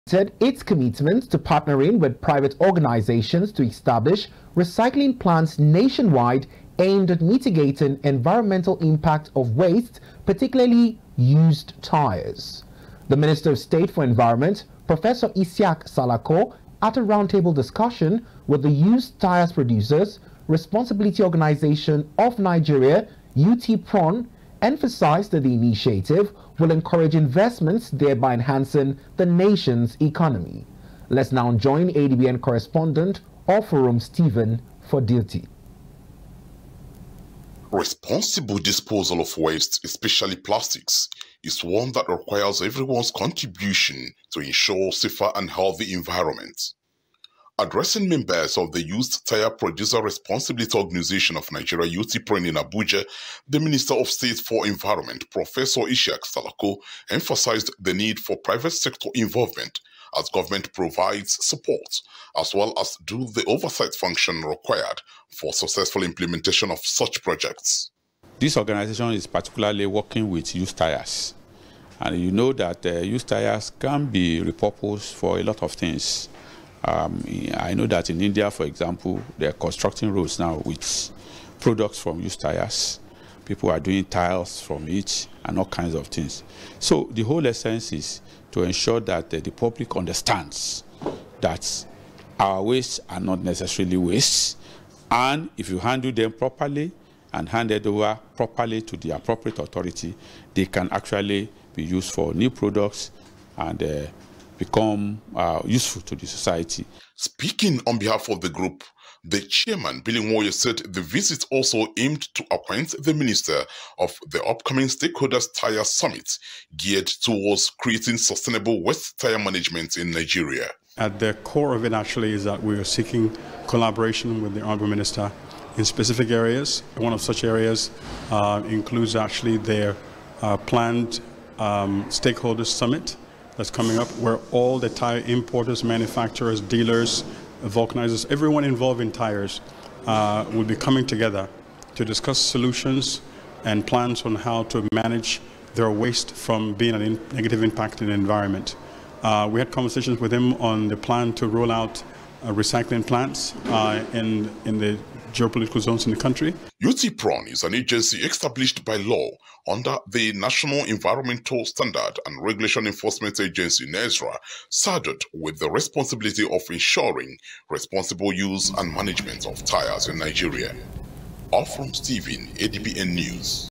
FG TO PARTNER PRIVATE SECTOR IN ESTABLISHING RECYCLING PLANTS
The federal government has reiterated it's resolve to collaborate with private organizations in the establishment of recycling plants across the country in order to reduce the environmental impact of wastes especially tyres that have reached their end of life cycle.

The minister of state for Environment Professor Iziak Salako at a roundtable with the Used Tyres Producer Responsibility Organization of Nigeria - UT-PRON - said the initiative will facilitate investment to boost the nation's economy.